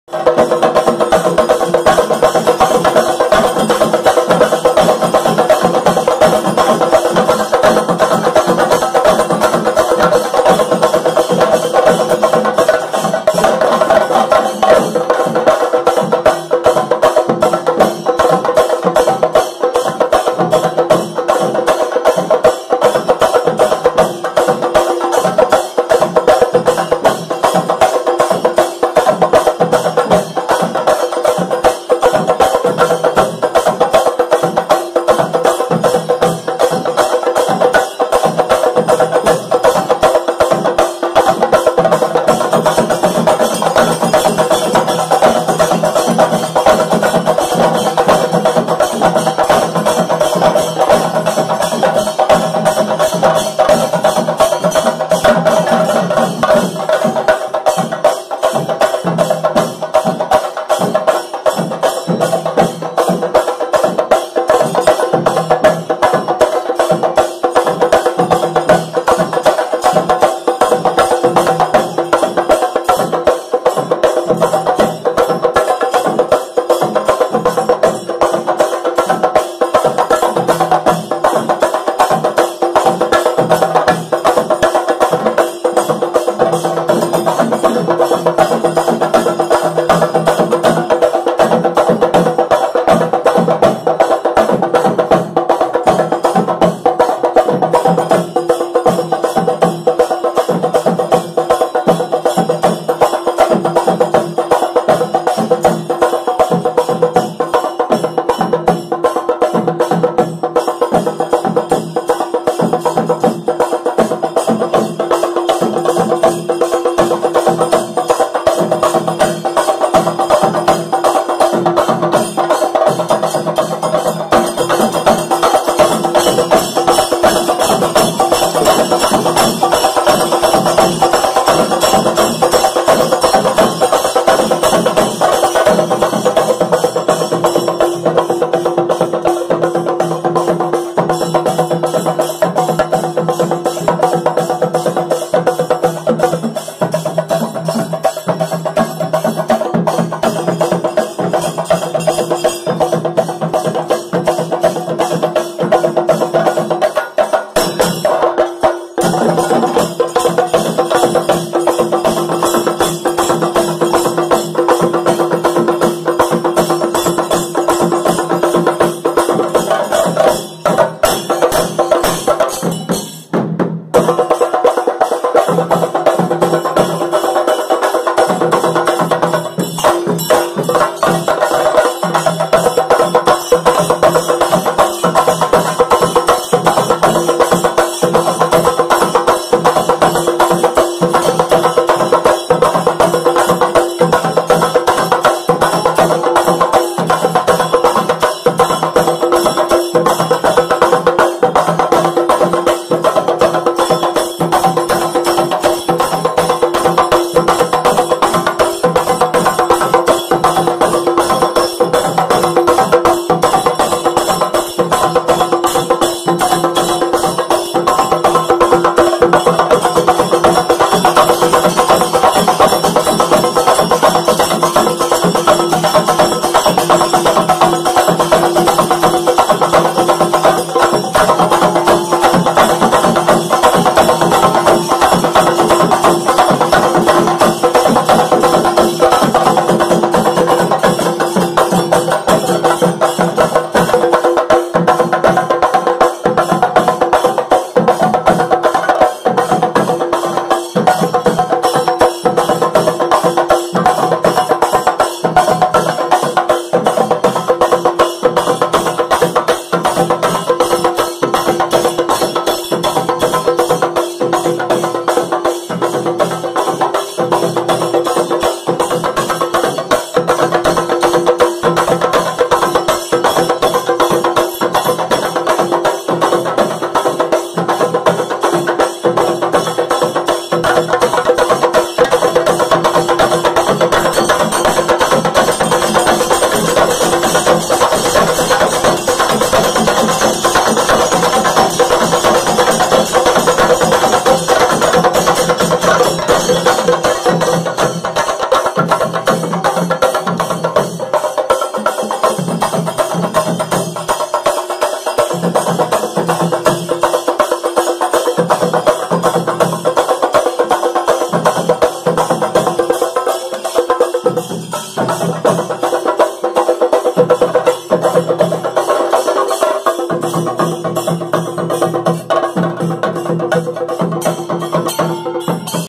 Folk Music By Kerala Team at Vontimitta Temple
తప్పక  చూడవలిసిన  వీడియో
ఒంటిమిట్ట  శ్రీ  కోదండరాముని  సన్నిది  తిరుణాల లో కేరళ  వారి సంగీత  ప్రదర్శన .

Must Watch video :

This music composed in Vontimitta temple on the occasion of tirunala.